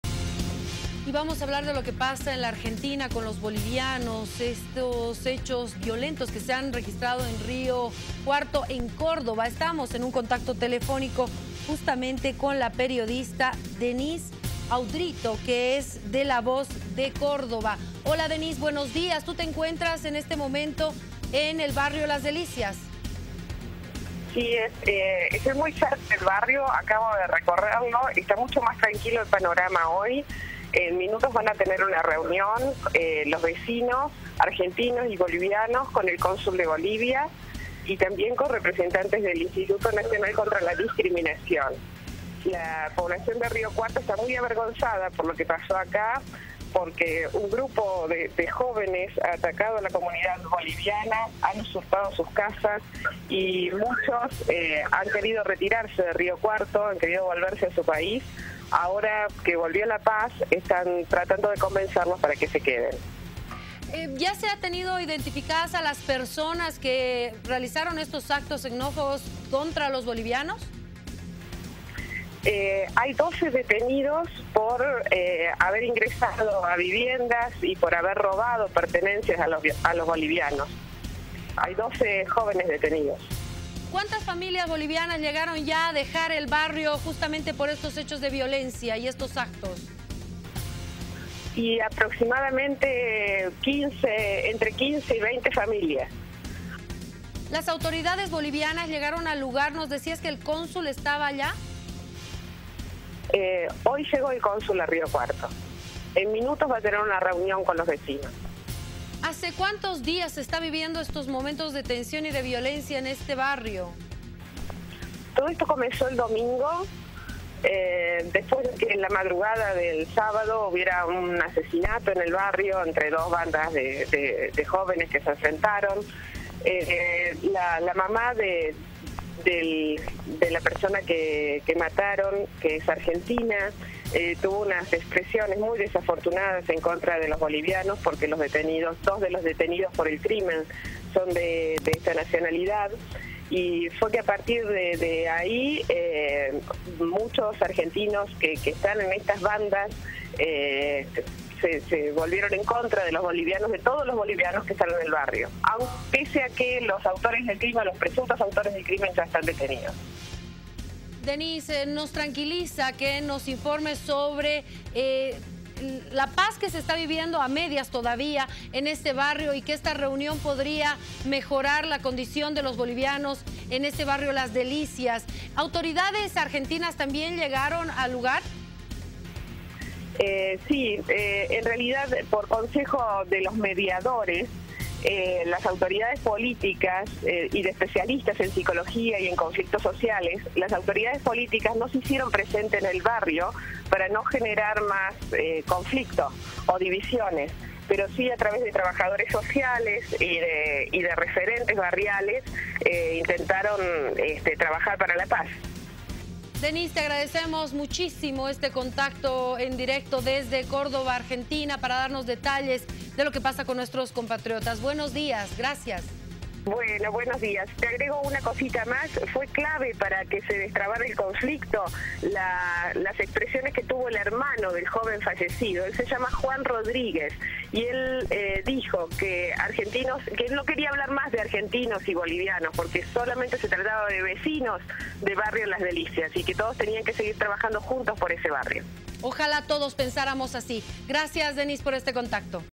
Levántate Bolivia con Denis Audrito Periodista de "La voz de Córdova"
Xenofobia contra bolivianos en Argentina